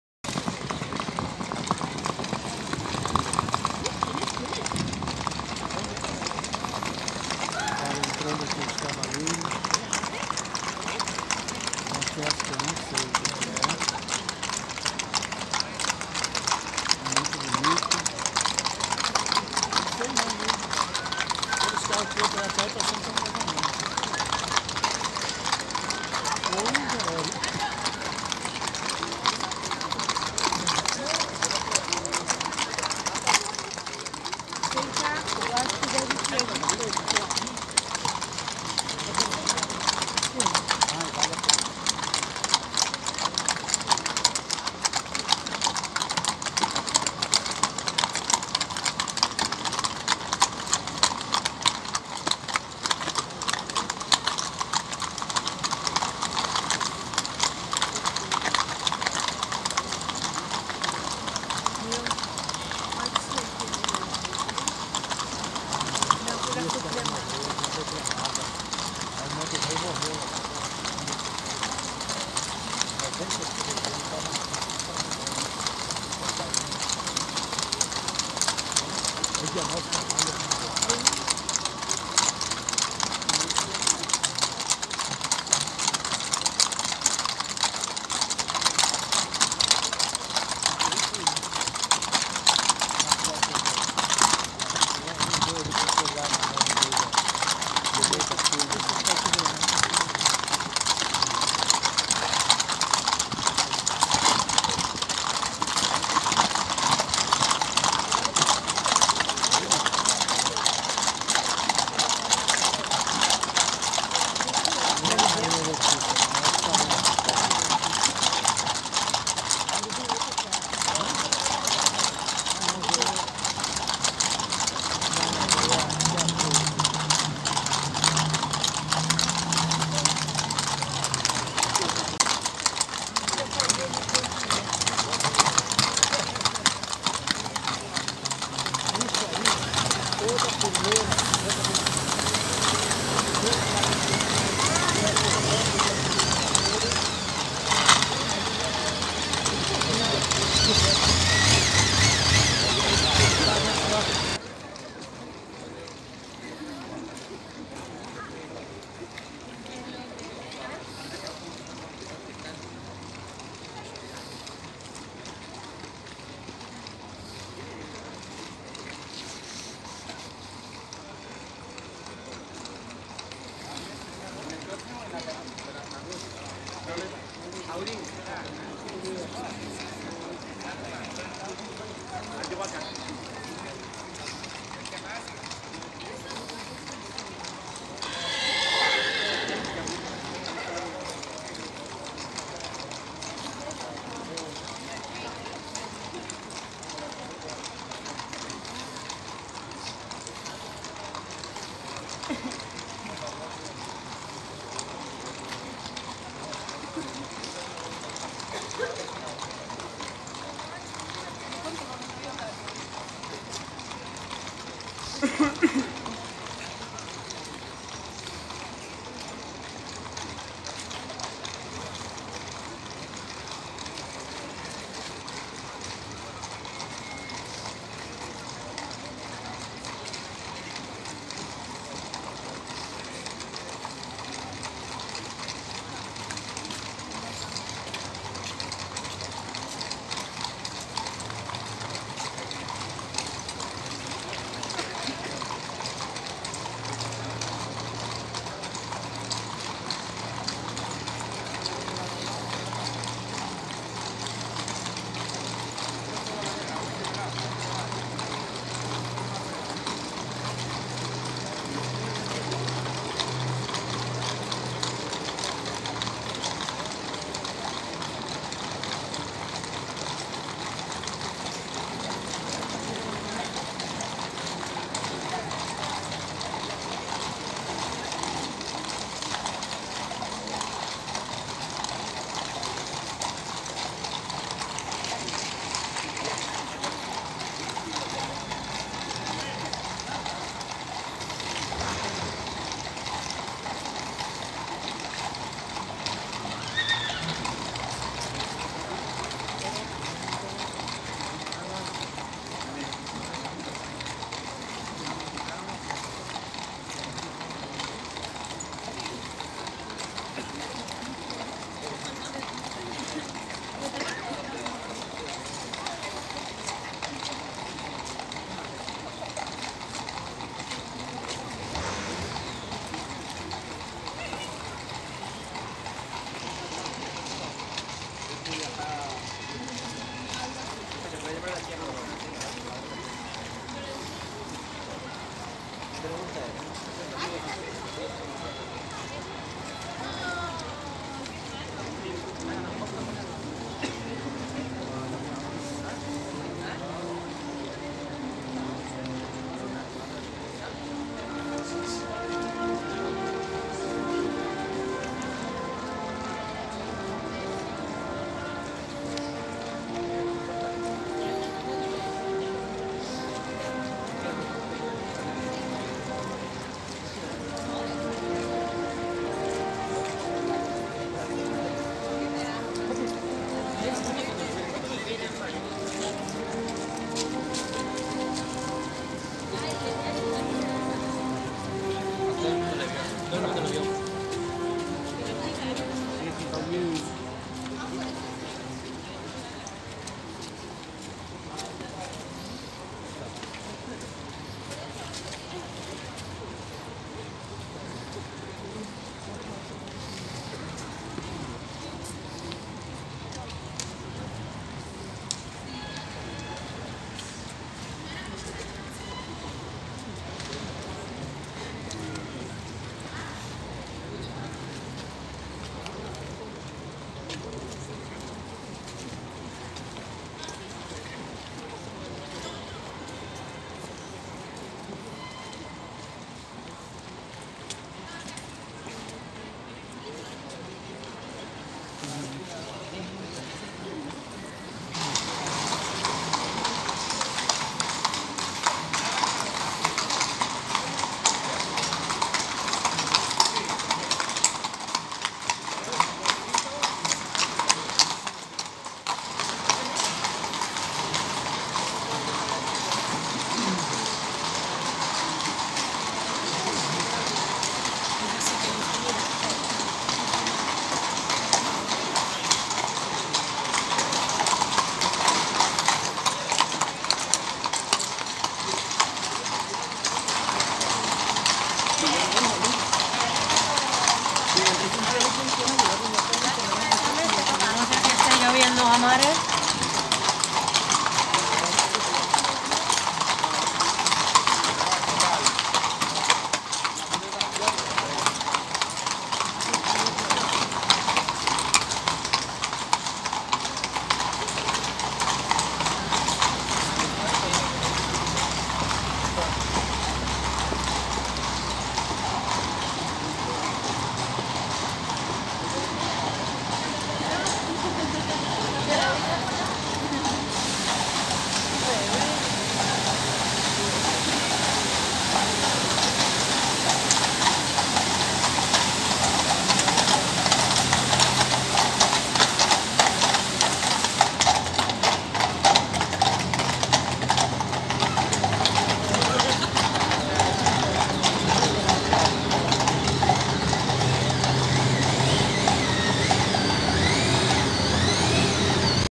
Comitivas para la Presentación de Cartas Credenciales en el Palacio Real de Madrid
Traslado de los Embajadores desde el Ministerio de Asuntos Exteriores de Madrid al Palacio Real para la Presentación de sus Cartas Credenciales.